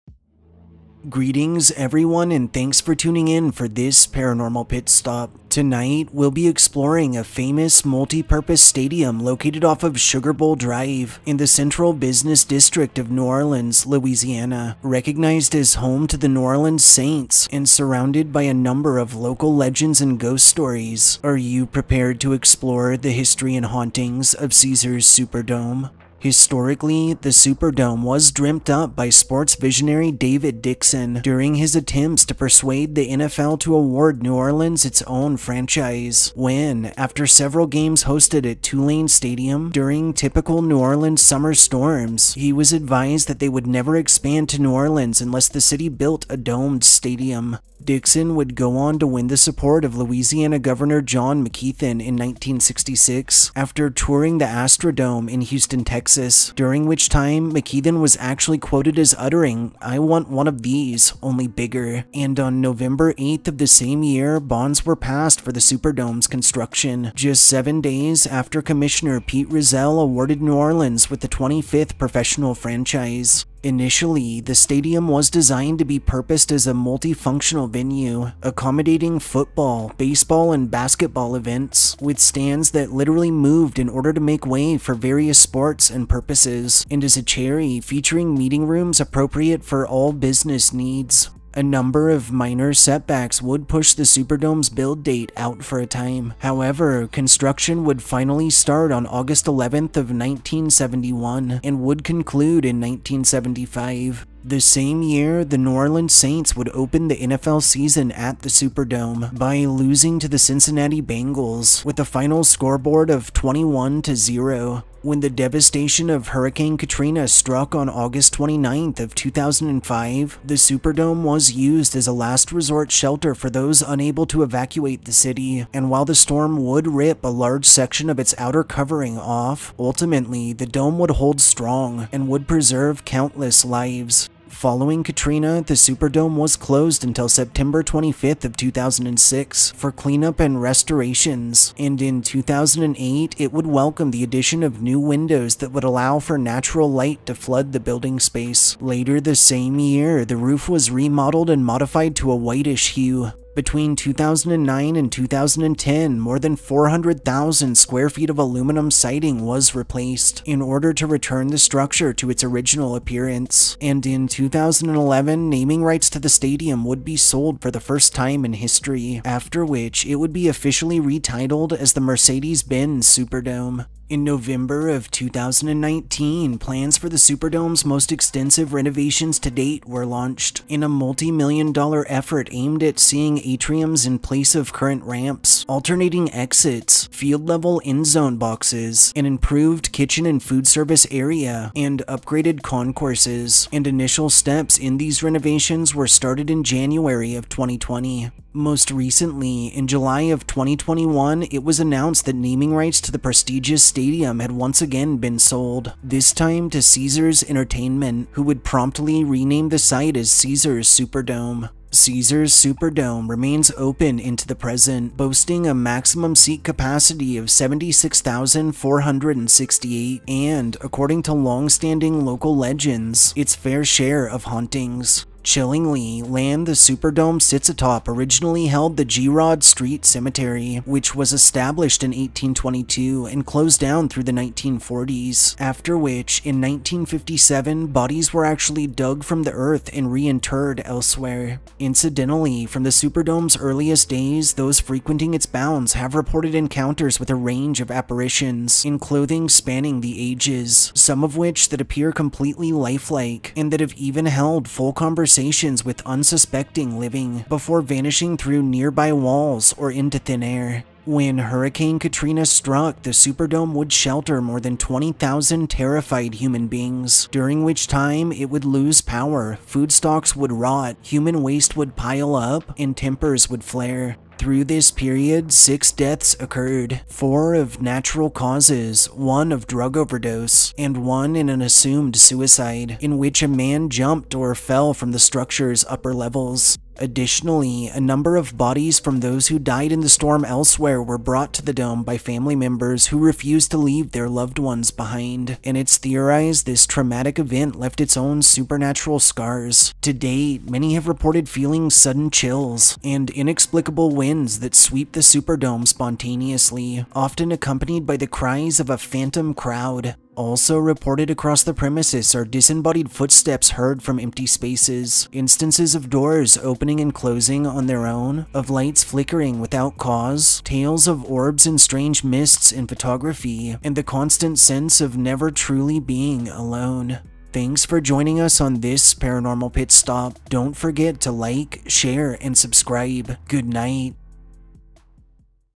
Haunting of the Caesars Superdome
Located in the heart of New Orleans, Louisiana, lies a famous, multi-purpose stadium recognized as home to the New Orleans Saints... that's shrouded in a number of ghost stories and local legends. Are you prepared to brave the history and hauntings of the Caesars Superdome?

"161125-M-RB239-009" by U.S. Department of Defense Current Photos is in the Public Domain